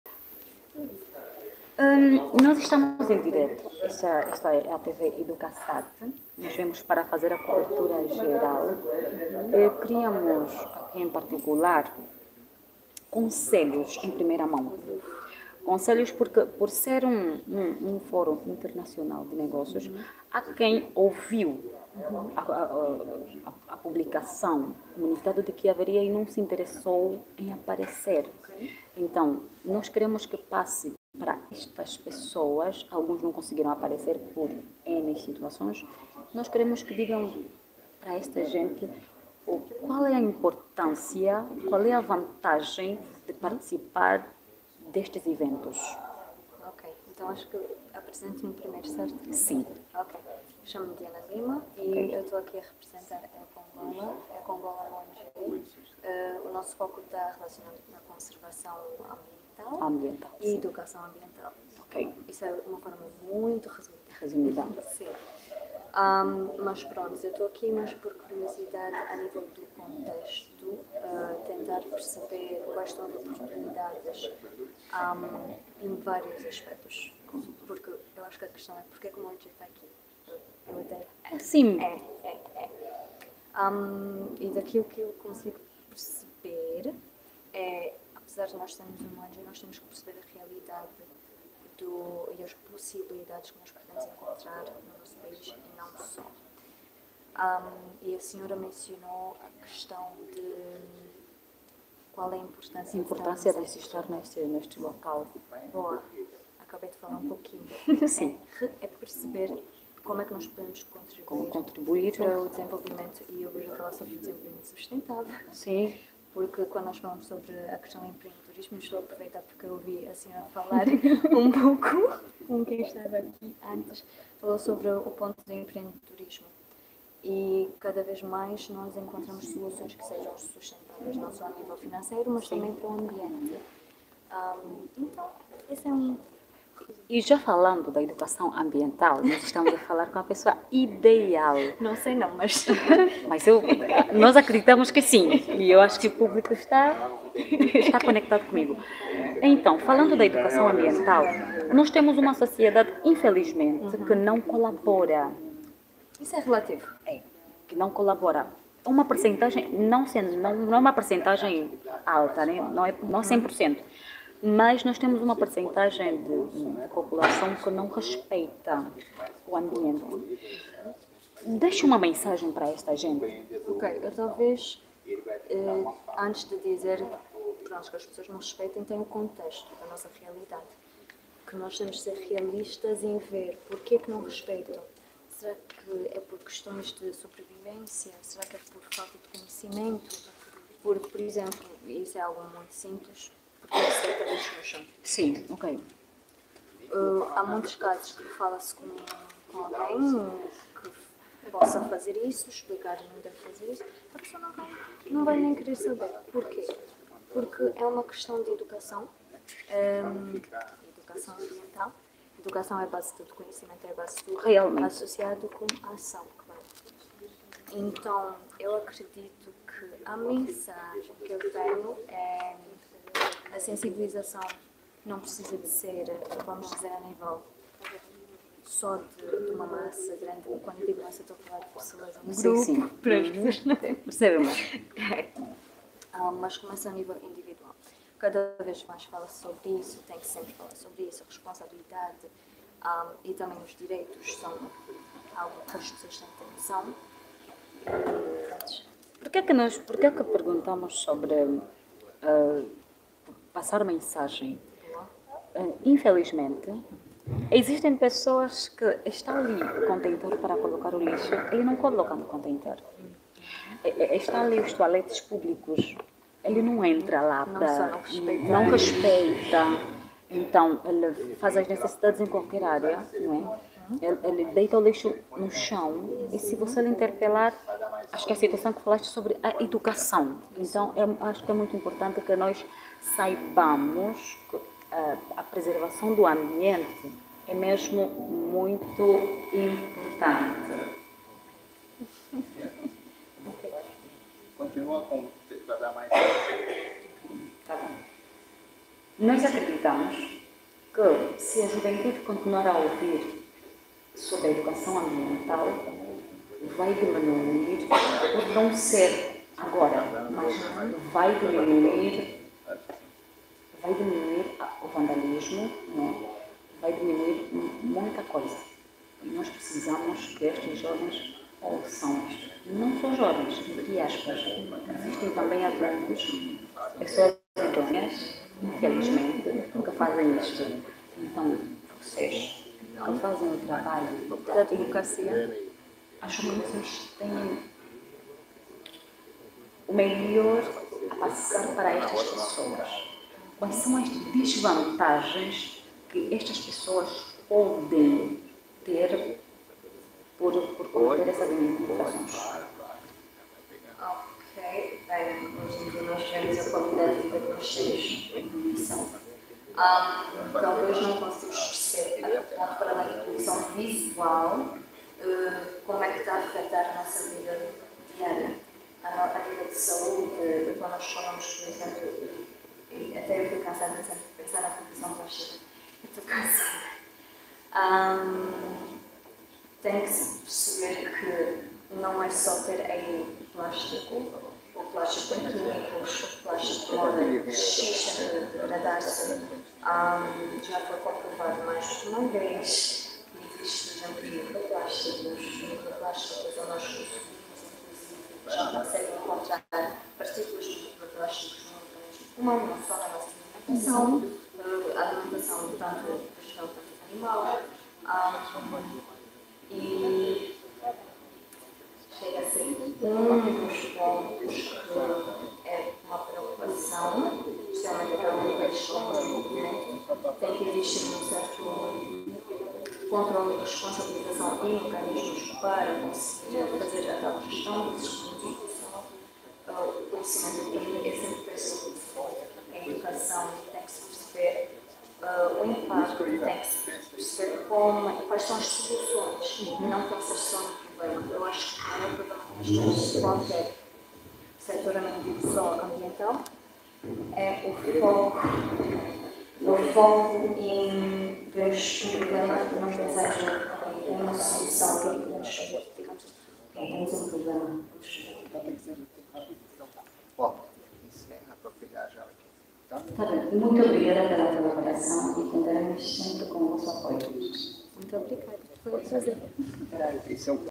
DIANA LIMA, ecoangola.com no IV IBFA.
Entrevistadora: Bantinda Glória Tomás Gomes - TV Educasat
Entrevistada: Diana Lima - Diretora Executiva Interina da ECOANGOLA
Local: Palácio da Justiça de Angola - IV IBFA - International Business Forum Angola
realizado no dia 29 de Junho de 2024.

Texto Inicial - Celso Salles, escritor afro-brasileiro, pesquisador, articulador cultural e esportivo, ativador de estratégias políticas e intercâmbio entre países africanos e o mundo.

Foi um momento muito importante, antes de iniciar o IV IBFA. Nada estava preparado. Entrevistadora e Entrevistada foram pegas de surpresa. A iluminação e o som local ainda estavam em ajustes, porém não poderíamos perder a oportunidade na transmissão AO VIVO que se iniciou rigorosamente no horário. Normalmente nestes eventos, tem muitas pessoas de importância capital e nem sempre conseguimos entrevistar todos. Peço desculpas pela qualidade da imagem e mesmo do som, mas não poderíamos em hipótese alguma perder esta oportunidade.
Dra. Diana Lima nos trouxe de presente um projeto MARAVILHOSO que pode ver em riqueza de detalhes na Plataforma Digital

A EcoAngola é uma Associação não Governamental, sem fins lucrativos, vocacionada para a promoção da sustentabilidade com objetivo de preservar o ambiente e promover o bem-estar social em Angola.
A EcoAngola organiza eventos, conferências e exposições, assim como organiza e apoia campanhas de limpeza de praias, campanhas de plantação de árvores, e outras campanhas que visem a preservação do ambiente e a promoção do bem-estar social. Nós promovemos e implementamos projectos ligados à sustentabilidade ambiental, financiados pelo sector público ou privado, por organizações especializadas ou como parte da responsabilidade social corporativa do sector privado.
A EcoAngola também organiza acções de formação e webinars em vários tópicos relacionados com sustentabilidade ambiental, direccionados às escolas, organizações públicas e privadas, e ao público em geral.